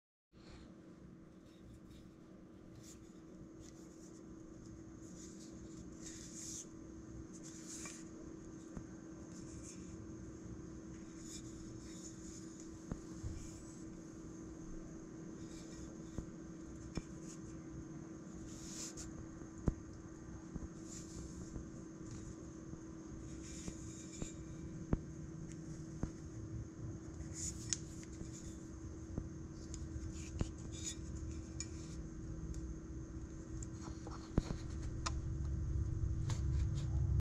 Large 250mm Japanse Nata Axe - Original Condition
This is a heavily used and very large Japanese Axe. a Nata Axe. This one is particularly large and heavy , and most likely quite old.  This is a short video clip about this axe, showing its original condition as i attempt to document restoring it to a very usable and functional condition. This axe weighs in at 3.3lbs. / 1450kg and with its very wide shape bevel , very strong.